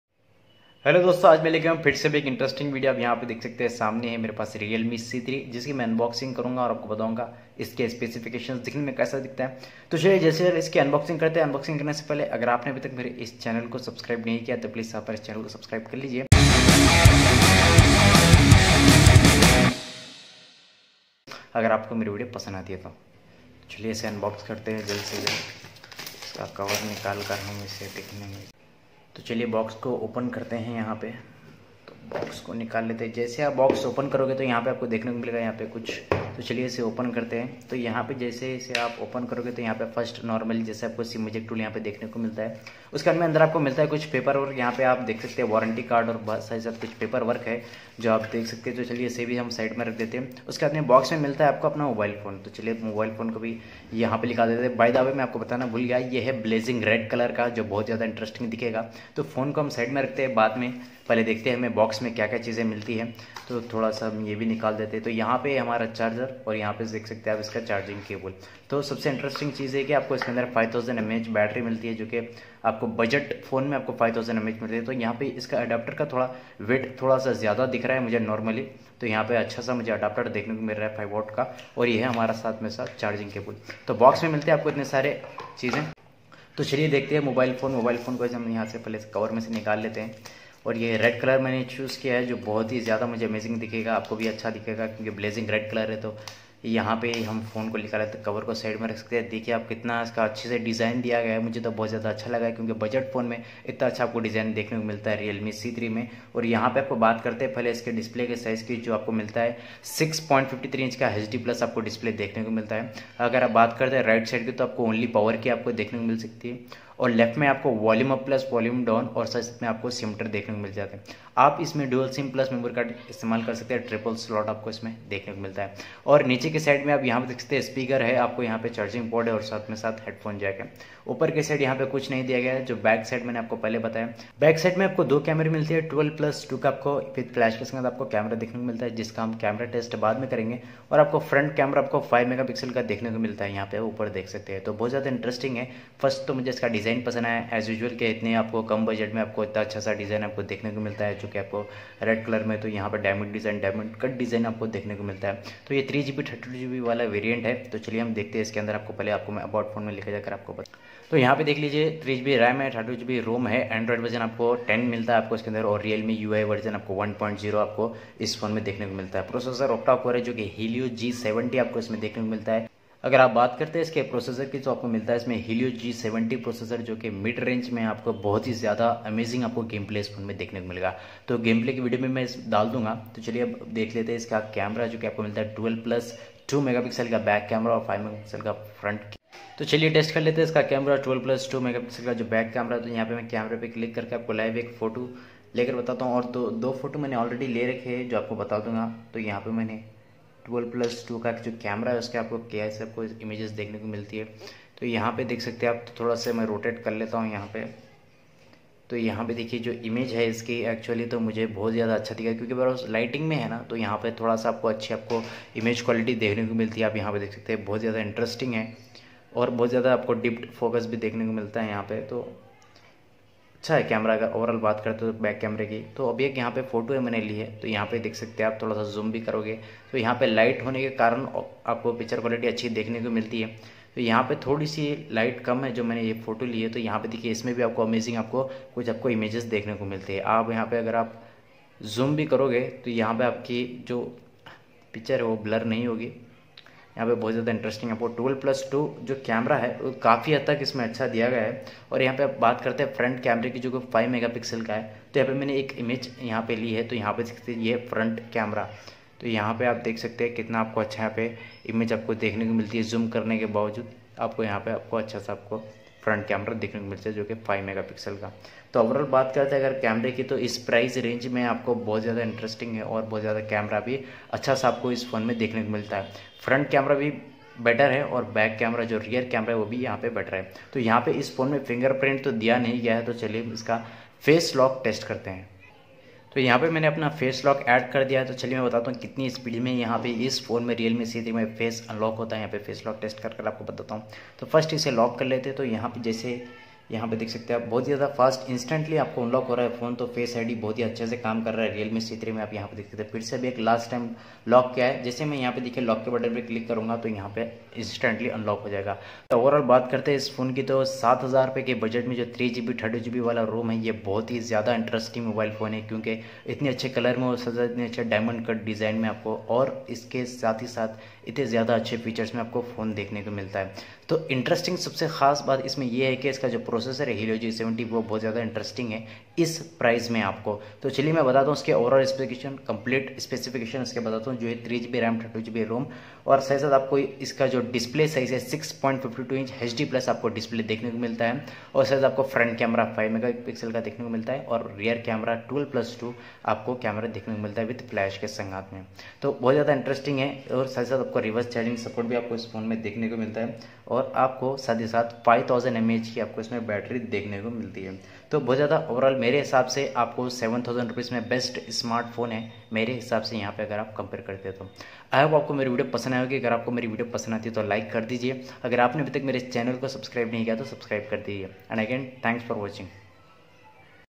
Realme C3 Unboxing And Review |Blazing Red Color
Hello Friends Now I am Back With Amazing Video  Now Today I am Unboxing Realme C3 Blazing Red Colour It's A Fabulous Device With Amazing Price, It Has Many Features Now You Can Play Amazing Games In this Phone With Hello G70 Processor For Better Understanding Watch My Video Till End  If You like My Video Then Please Subscribe My Channel And Thanks For Watching.
Price=6999. 3GB+32GB